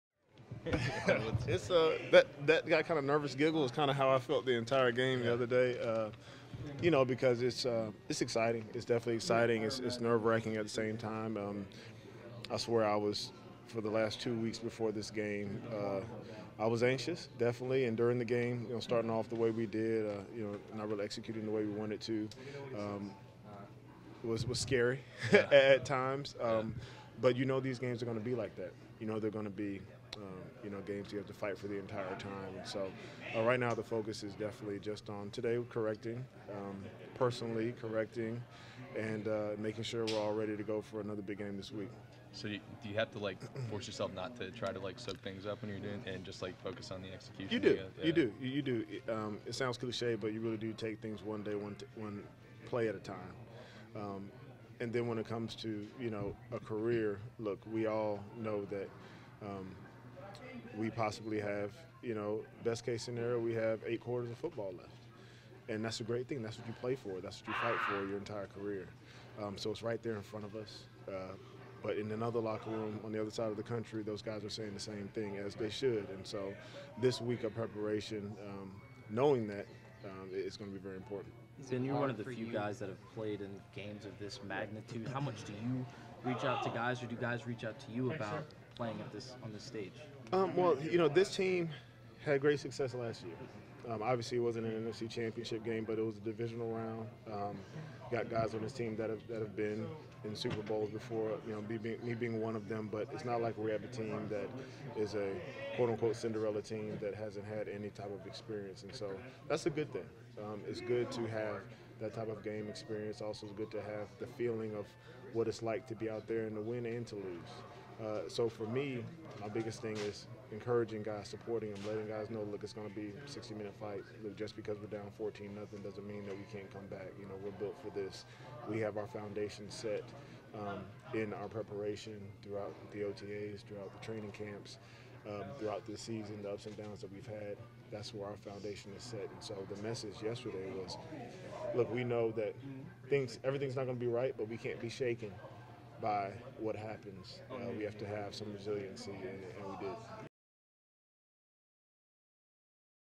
With 1 or 2 games left in his career, Ben Watson feeling excited, anxious, nervous
New Orleans Saints veteran tight end Ben Watson announced a few weeks ago that this would be his last season in the NFL. With the Saints in the NFC Championship Game this weekend against the Rams, Watson talks about all the emotions he has been feeling.